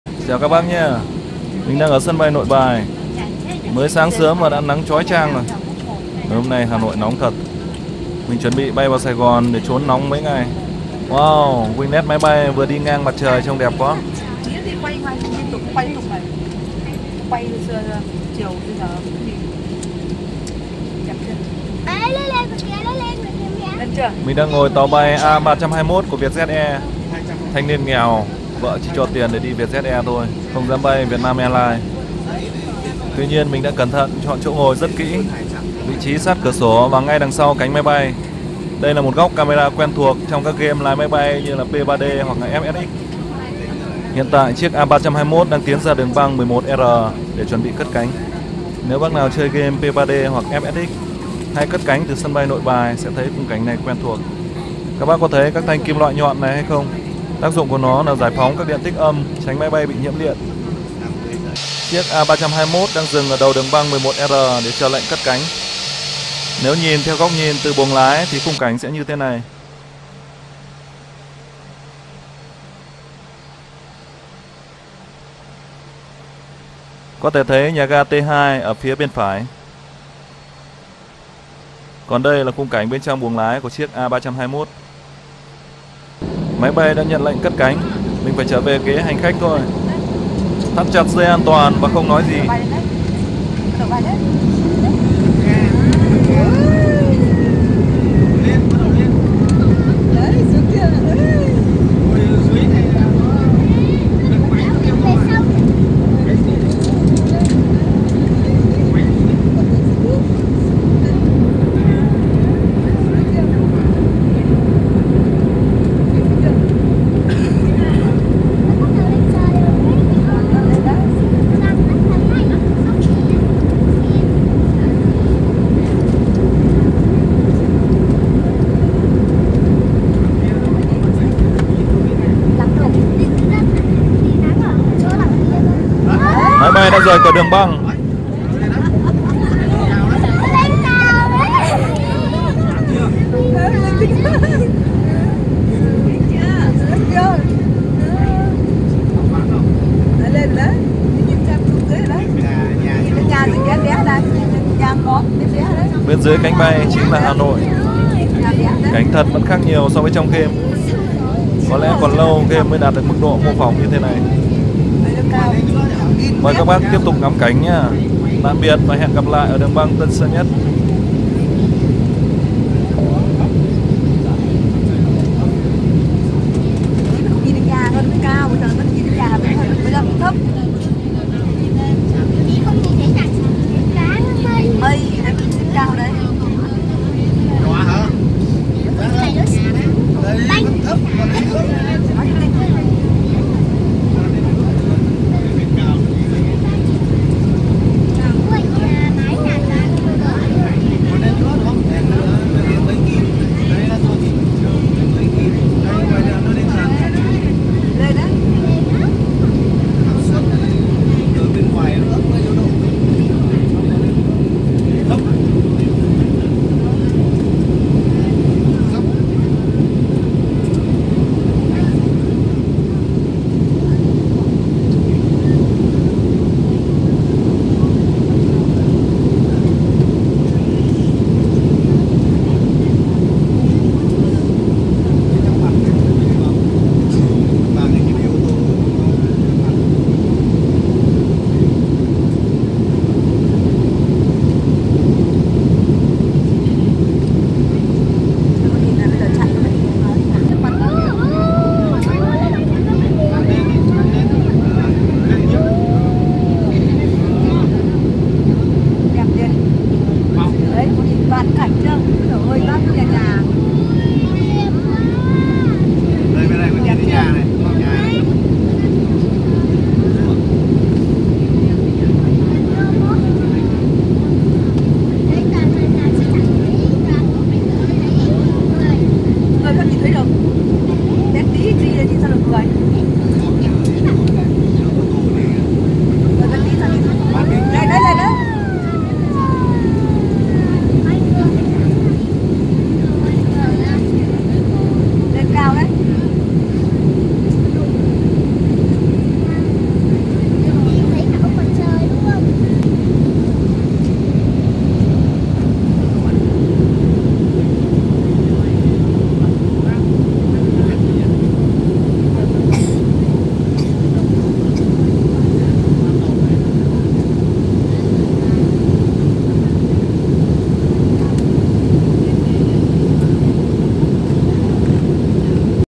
Máy bay A320 cất cánh từ sân bay Nội Bài (VVNB)
Máy bay Airbus A320 của Vietjet air cất cánh từ sân bay Nội Bài (VVNB) hành trình đến sân bay Tân Sơn Nhất (VVTS)